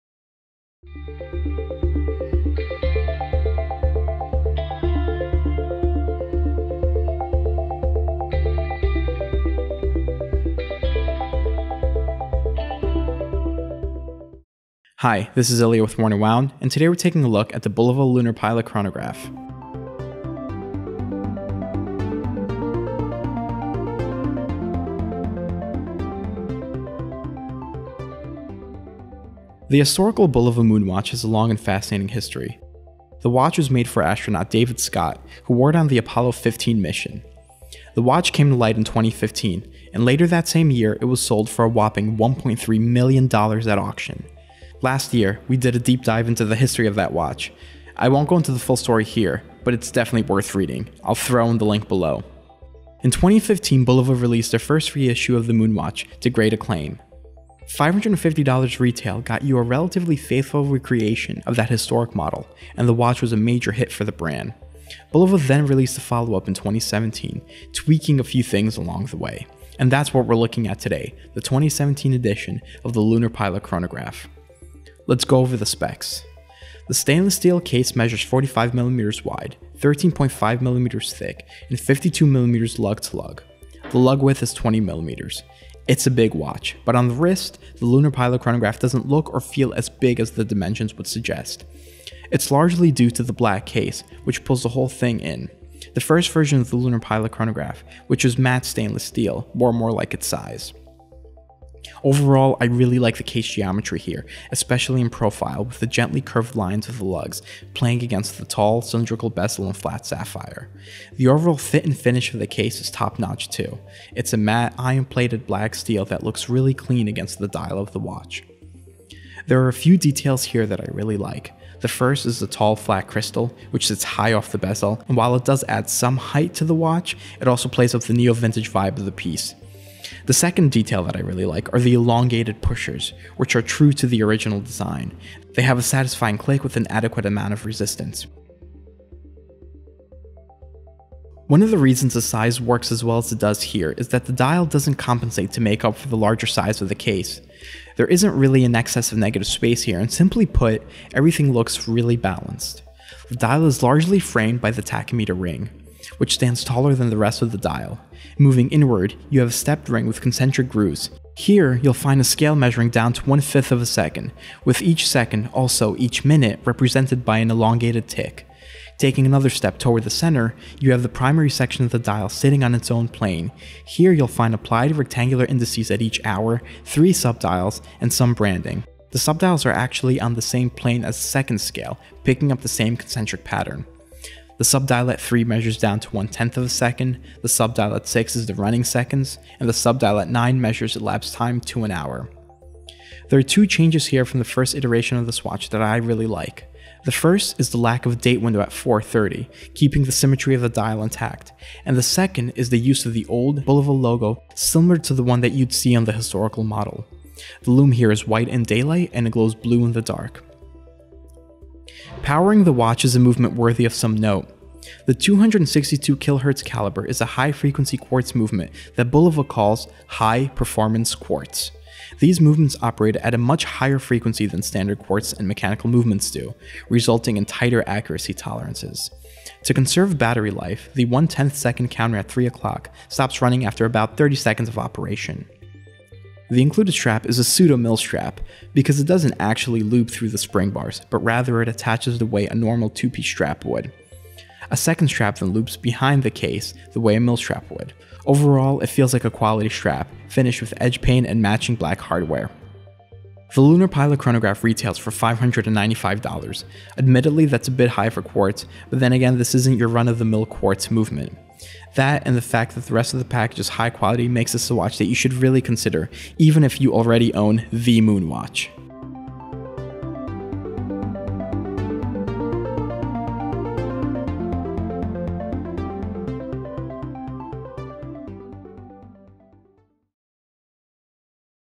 Watch Review: The Other Moon Watch—Bulova Lunar Pilot Chronograph 2017
Today, we take a look at the 2017 edition of the Bulova Lunar Pilot Chronograph. For the article mentioned in the video,